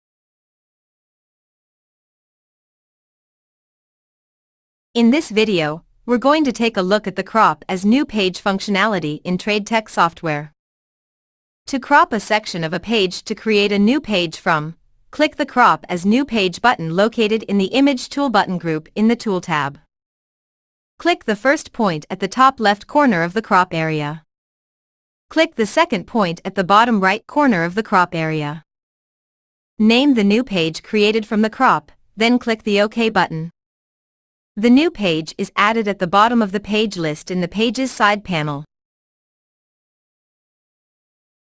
Crop as New Page
How to create a new page from the crop of an existing page in TradeTek Takeoff & Estimating software.